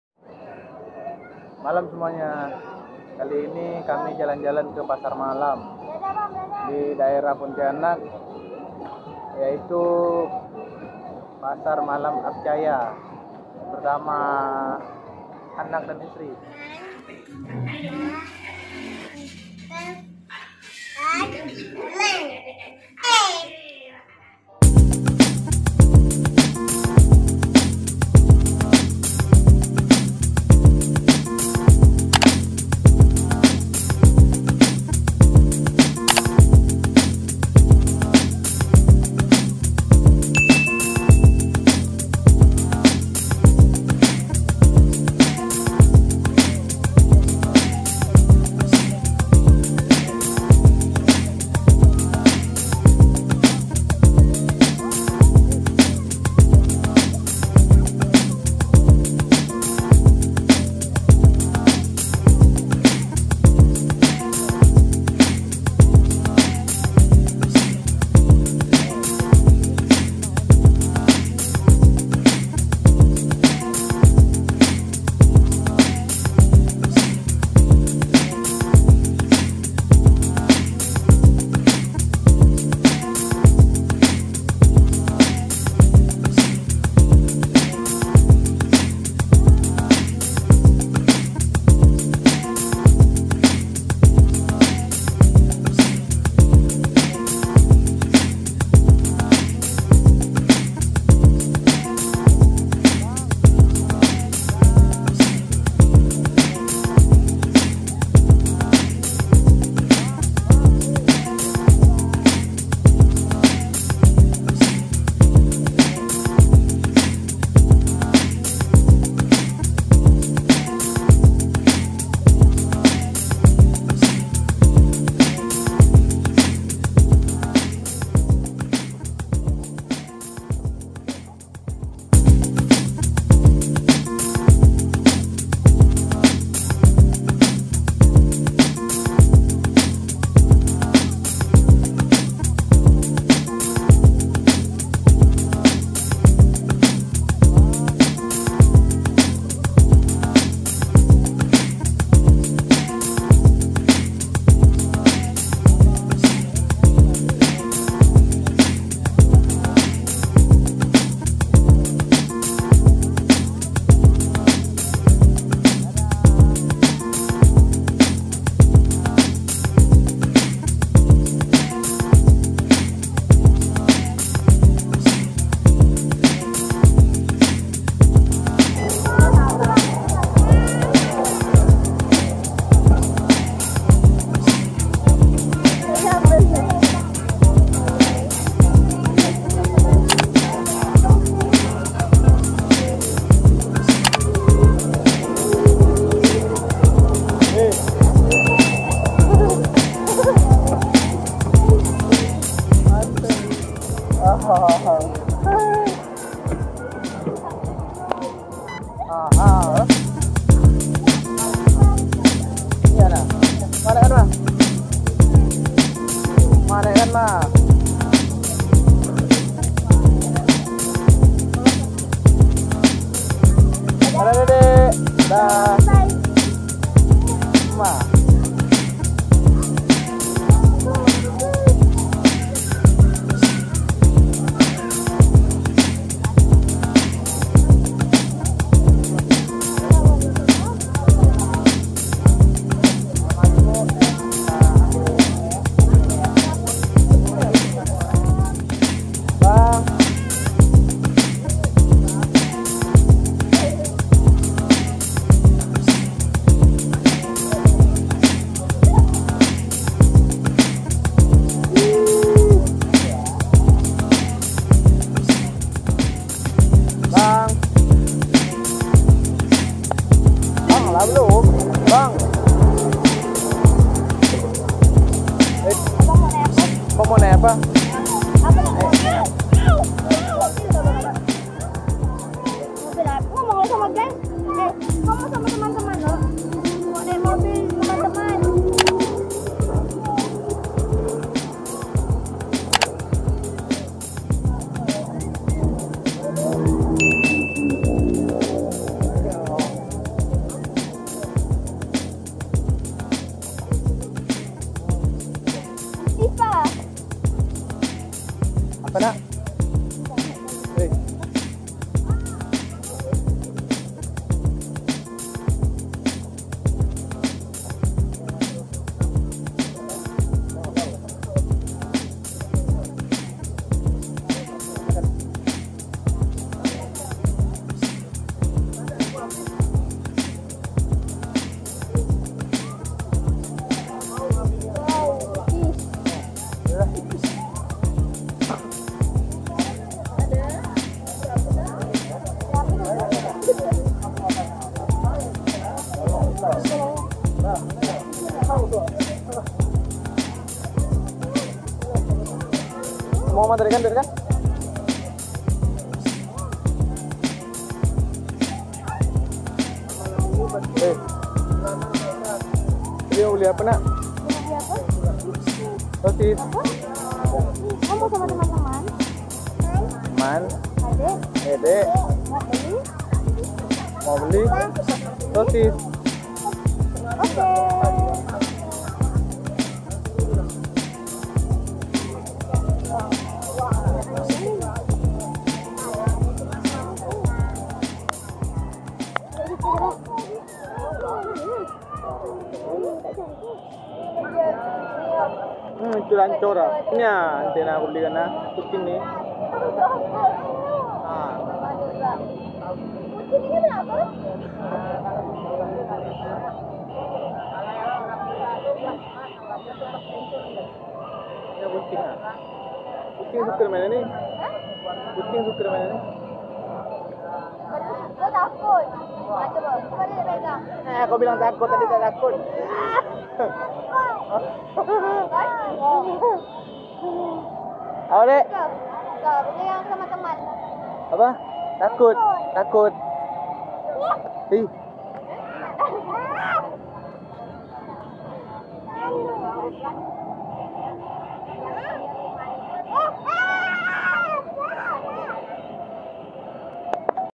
Dek Azriel main kereta dipasar malam || Azriel played the train in the night market
Saksikan keseruan dedek azriel main kereta main pesawat dan jalan jalan ke pasar malam bareng mama dan papa